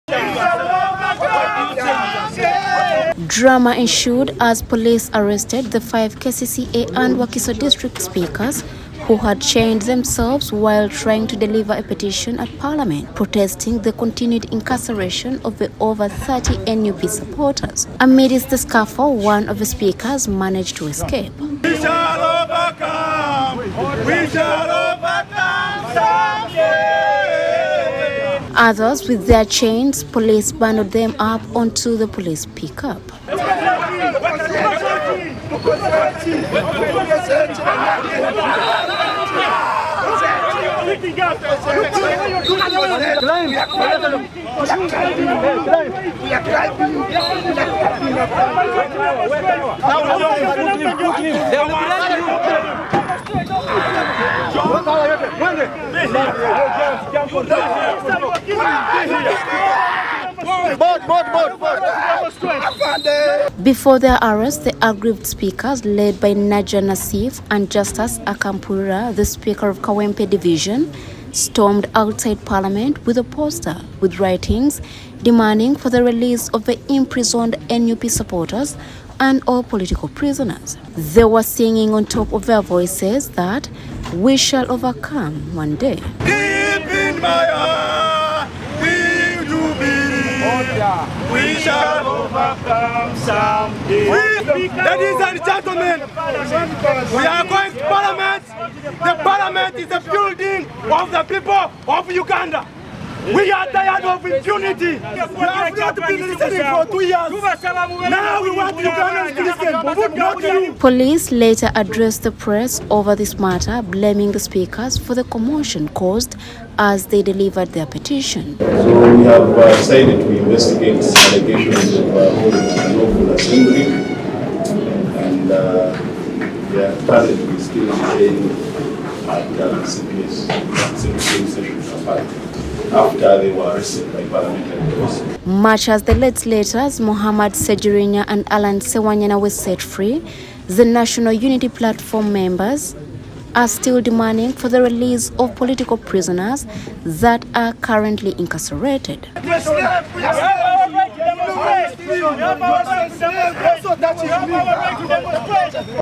Chaos as police arrests KCCA speakers
Drama ensued outside parliament today as police arrested the 5 division speakers from both KCCA and wakiso district; the speakers had chained themselves protesting the continued incarceration of NUP supporters.